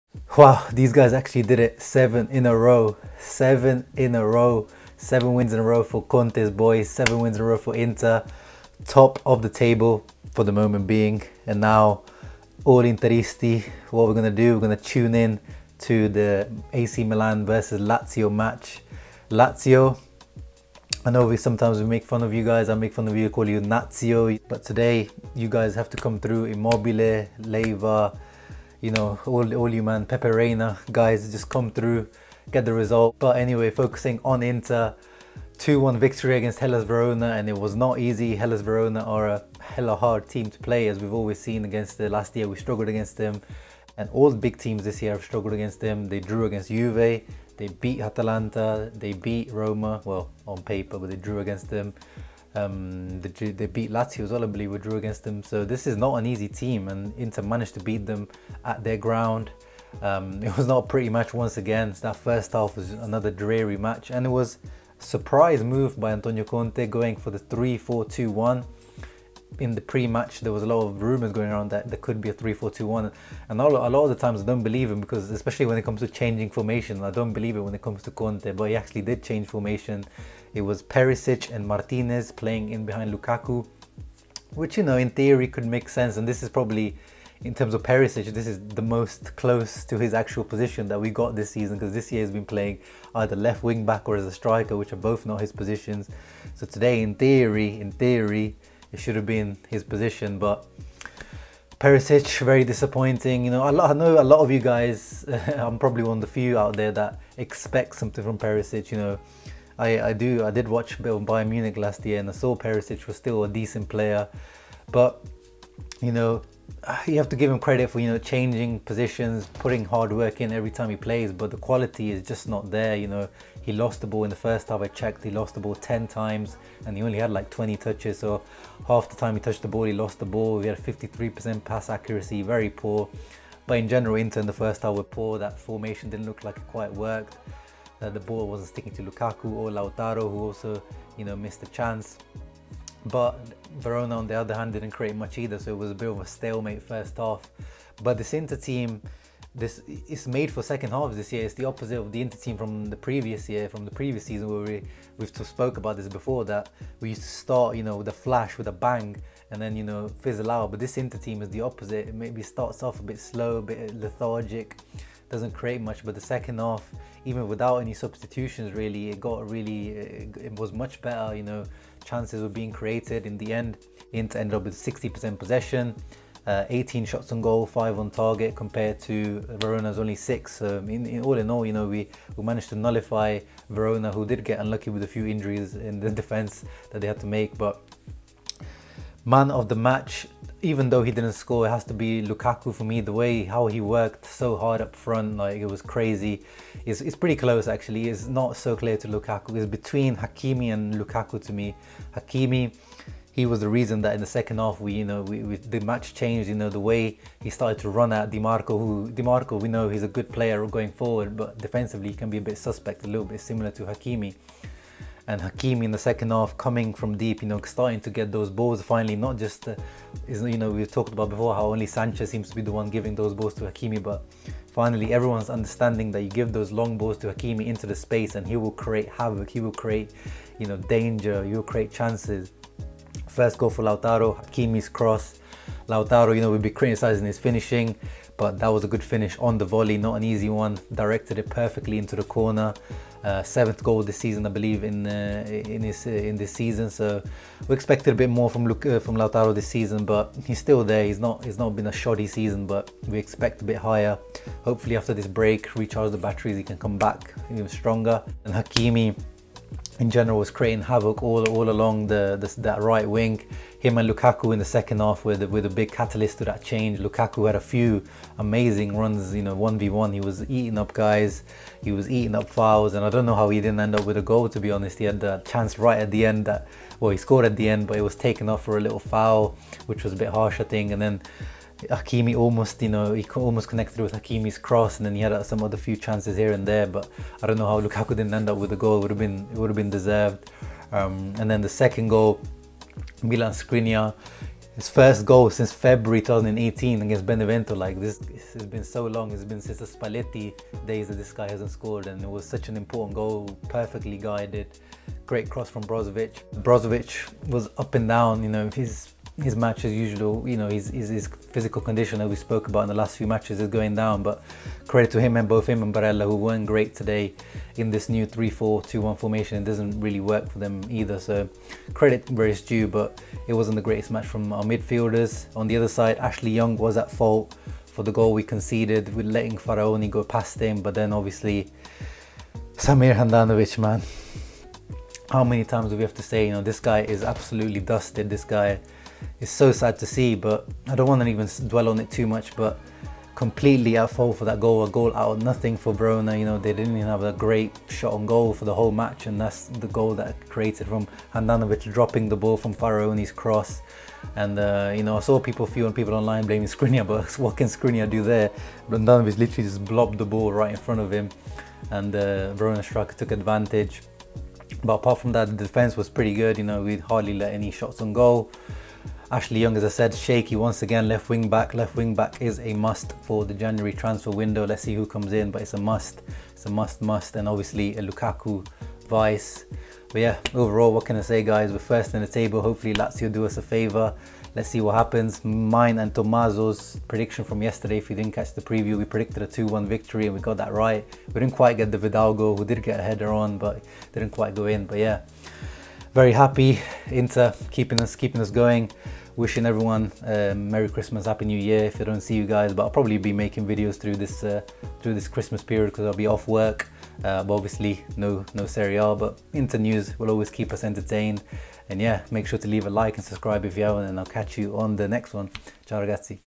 VERONA 1 - 2 INTER | Match Reaction | 7 IN A ROW! | Interisti Can Have a Peaceful Christmas
7 In a row means Interisti can have peaceful holidays with their family...